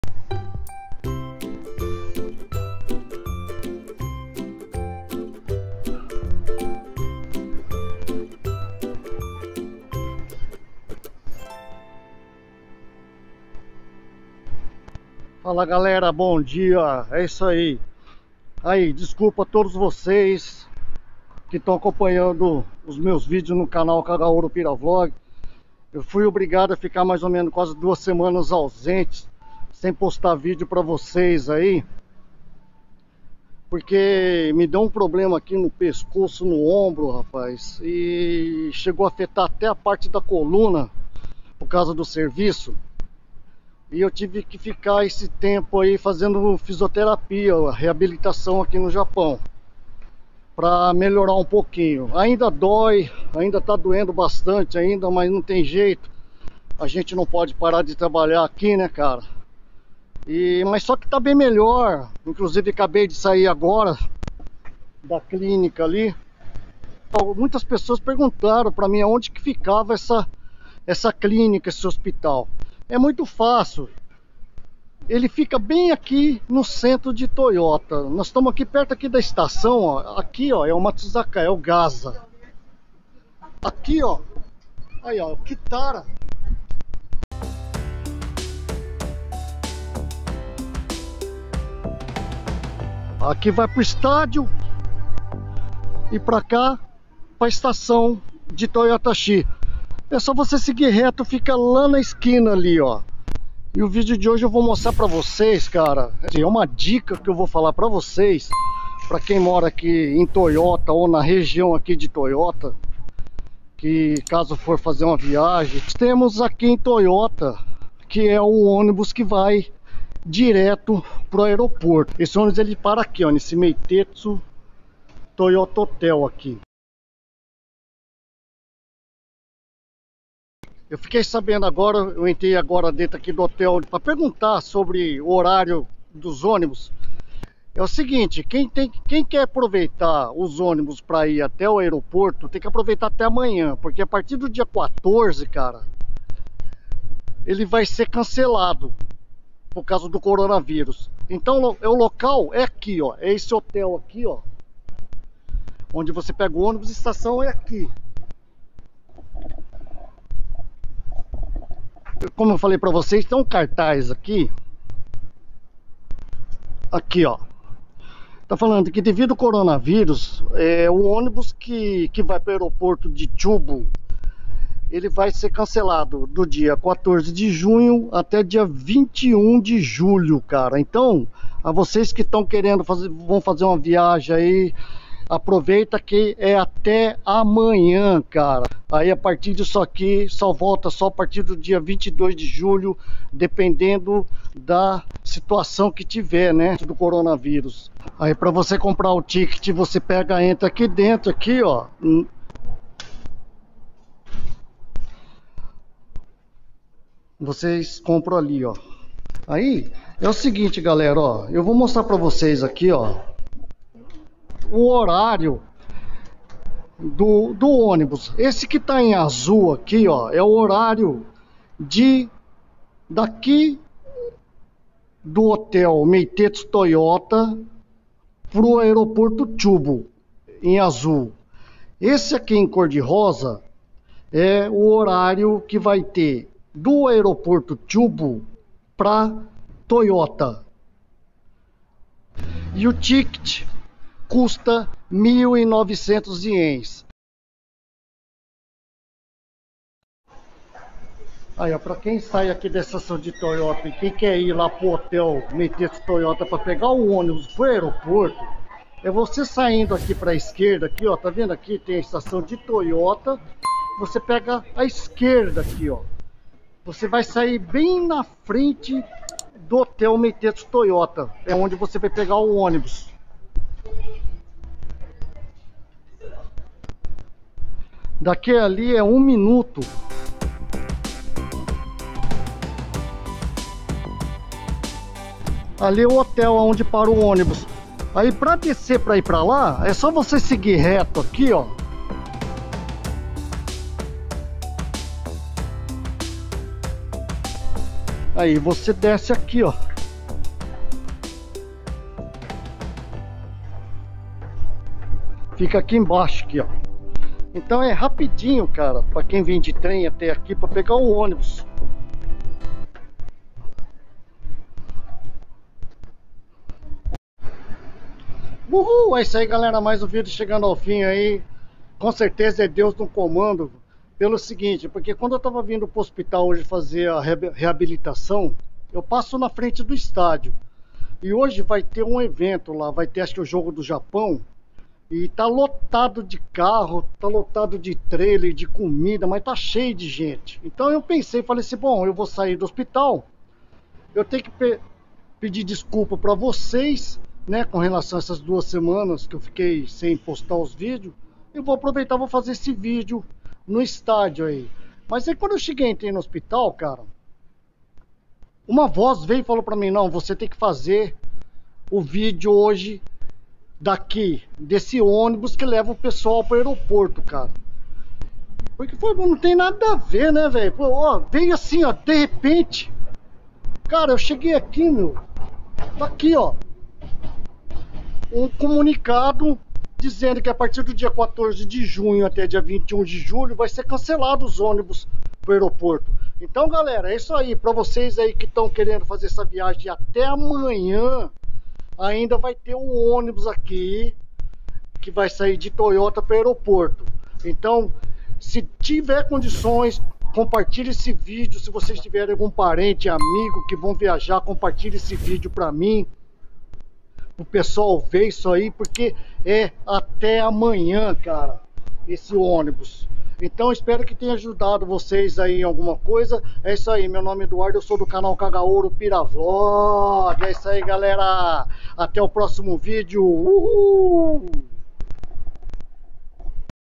GALERA.. PERDÃO🙏 + ALERTA!! CANCELAMENTO DO ÔNIBUS DE TOYOTA PARA AEROPORTO CHUBU (12/06/2021)🎌
Esse vídeo seria uma dica, mas acabou sendo um ALERTA!! cancelamento do ônibus de TOYOTA para o AEROPORTO CHUBU ( Tokuname ), ao mesmo tempo gostaria de pedir mil perdoes por esses 2 semanas que deixei de postar vídeos para vocês... me deu um problema no pescoço, ombro e coluna tive que fazer reabilitação por 2 semanas, estou voltando agora aos poucos, vou continuar a reabilitação agora será 2 vezes por semana... logo estarei em plena forma se DEUS quiser obrigado a todos e por favor não me abandonem 🙏🙏

This video would be a tip, but it turned out to be an ALERT!! cancellation of the bus from TOYOTA to CHUBU AIRPORT (Tokuname), at the same time I would like to lose a thousand pardons for these 2 weeks that I stopped posting videos for you... I had a problem with my neck, shoulder and spine I had to do rehabilitation for 2 weeks, I'm coming back little by little, I will continue the rehabilitation now it will be twice a week... soon I'll be in top shape if GOD wants thank you all and please don't leave me 🙏🙏

Meitetsu Toyota Hotel (名鉄トヨタホテル)
Localizado em: Gaza
Endereço: 1 Chome-140 Kitamachi, Toyota, Aichi 471-0027
consultas Meitetsu bus Toyota vendas de bilhetes telefone (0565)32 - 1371
Preço da passagem 1.900 Yen por pessoa

ATENÇÃO: Cancelamento do dia 14 de Junho até 21 de Julho de 2021